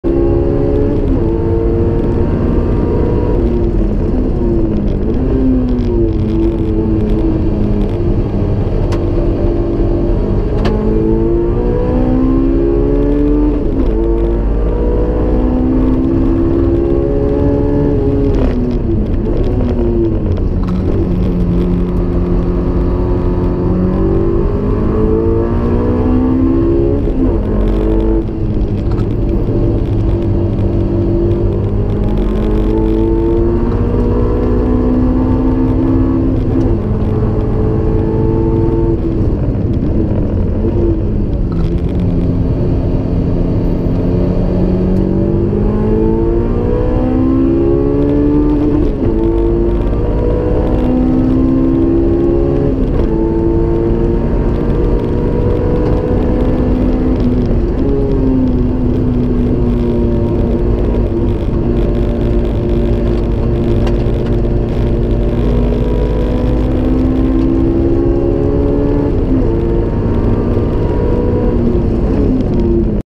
筑波 TC2000 走行動画 S2000 AP1
こんにちは！MANABOONです(＾∀＾)
本日の動画は、筑波TC2000の走行動画になります。
初のTC2000なので、全然ダメですが、これから徐々にタイムアップ目指していきます(´▽｀)

タイム【1:07.798】

・タイヤ NANKANG CR-S

MANABOONではYouTubeにて、在庫紹介＆マフラー音、FUJI-1GPや、走行会等の走行動画等をUPしております☆